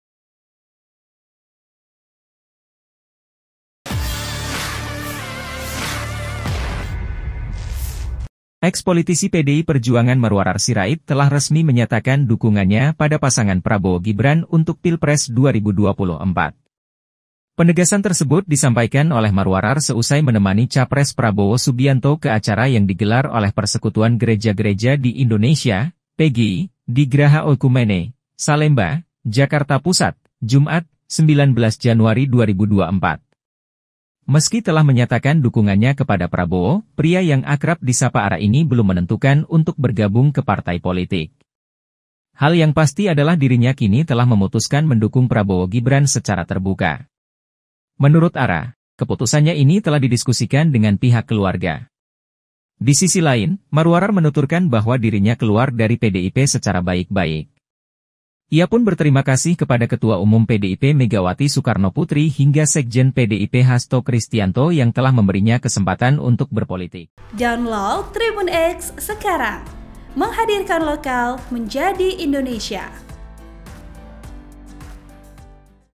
Maruarar Belum Putuskan untuk Gabung Partai Politik Seusai Hengkang dari PDIP
Eks politisi PDI Perjuangan Maruarar Sirait telah resmi menyatakan dukungannya pada pasangan Prabowo-Gibran untuk Pilpres 2024.

Penegasan tersebut disampaikan oleh Maruarar seusai menemani capres Prabowo Subianto ke acara yang digelar oleh Persekutuan Gereja-gereja di Indonesia (PGI), di Graha Oikumene, Salemba, Jakarta Pusat, Jumat (19/1/2024).

Meski telah menyatakan dukungannya kepada Prabowo, pria yang akrab disapa Ara ini belum menentukan untuk bergabung ke partai politik.